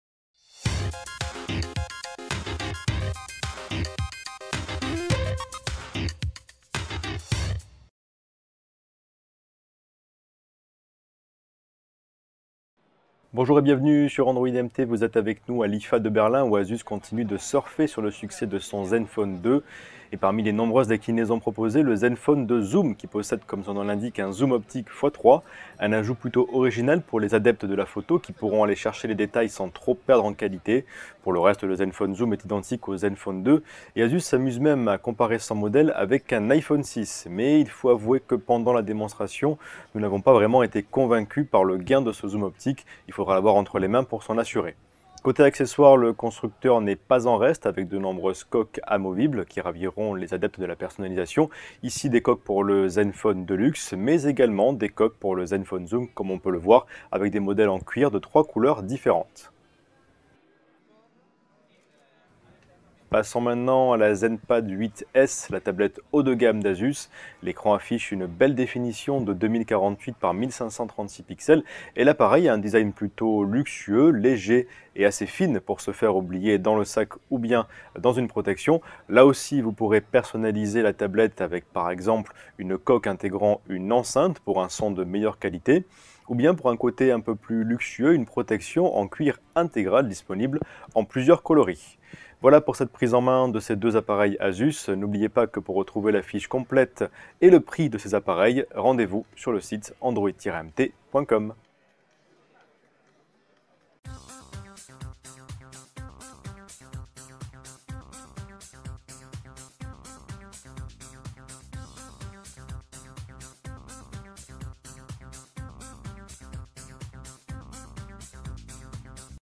IFA 2015 - Prise en main de l'ASUS ZenFone 2 Zoom et de la ZenPad 8 S
... Pour retrouver encore plus d'astuces, de tests et de tutoriels sur l'univers Android.

- IFA 2015 - La relève de l’Asus Zenfone est assurée avec le ZenFone 2 Deluxe

Asus surfe sur le succès de son ZenFone 2 en annonçant toute une série de déclinaisons : selfie, laser, zoom... C'est ce dernier que nous avons pris en main à l'IFA de Berlin, avec son zoom optique x3. Dans le même temps, nous faisons un tour d'horizon de l'Asus ZenPad 8 S, tablette musclée à petit prix, ainsi que de la gamme d'accessoires disponibles chez le constructeur taïwanais.